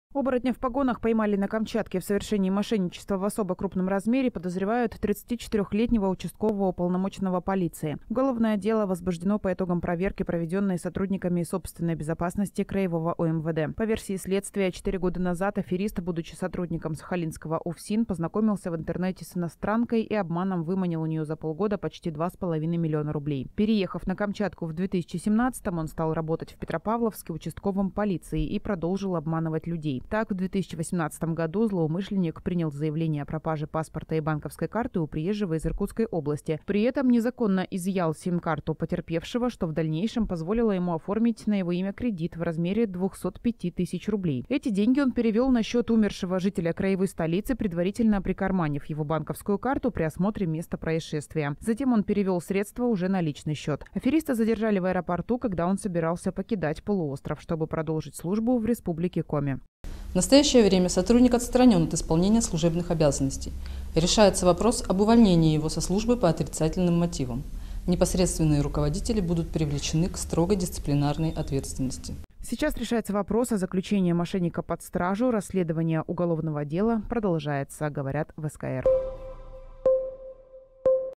«Оборотня в погонах» поймали на Камчатке | Новости сегодня | Происшествия | Масс Медиа
«Оборотня в погонах» поймали на Камчатке. В совершении мошенничества в особо крупном размере подозревают 34-летнего участкового уполномоченного полиции. Уголовное дело возбуждено по итогам проверки, проведенной сотрудниками собственной безопасности краевого УМВД. По версии следствия, 4 года назад, аферист, будучи сотрудником сахалинского УФСИН, познакомился в интернете с иностранкой, и обманом выманил у нее за полгода почти 2,5 миллиона рублей. Переехав на Камчатку в 2017-м, он стал работать в Петропавловске участковым полиции, и продолжил обманывать людей. Так, в 2018 году злоумышленник принял заявление о пропаже паспорта и банковской карты у приезжего из Иркутской области, при этом незаконно изъял сим-карту потерпевшего, что в дальнейшем позволило ему оформить на его имя кредит в размере 205 тысяч рублей. Эти деньги он перевел на счет умершего жителя краевой столицы, прикарманив его банковскую карту при осмотре места происшествия. Затем он перевел средства уже на личный счет. Афериста задержали в аэропорту, когда он собирался покидать полуостров, чтобы продолжить службу в  Республике Коми.

Сейчас решается вопрос о заключении мошенника под стражу. Расследование уголовного дела продолжается.